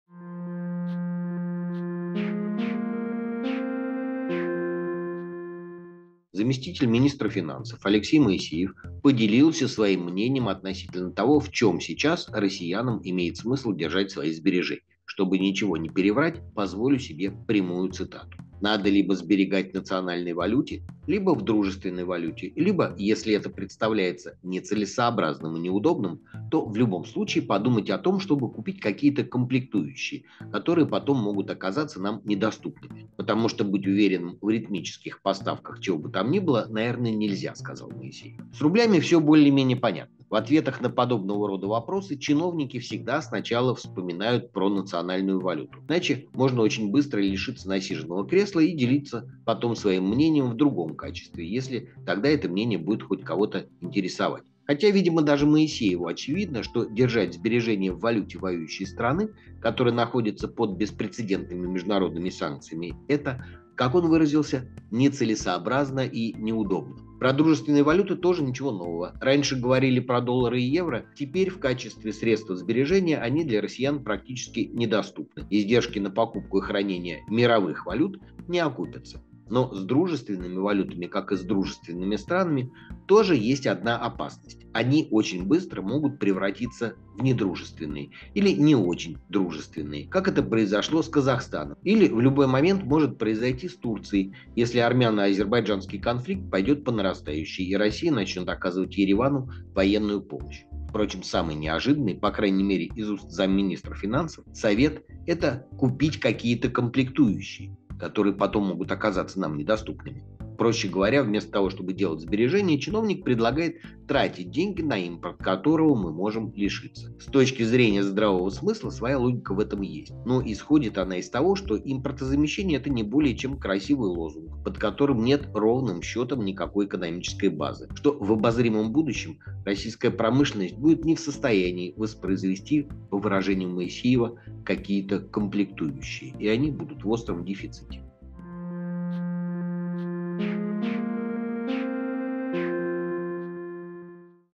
Вы не ждали, но вам дали
Новость дня.
Заместитель министра финансов Алексей Моисеев поделился советами относительно того, в чем сейчас россиянам имеет смысл держать свои сбережения.
Максим Блант коротко отвечает на вопросы о деньгах сегодня в рамках проекта "Деньги на Свободе".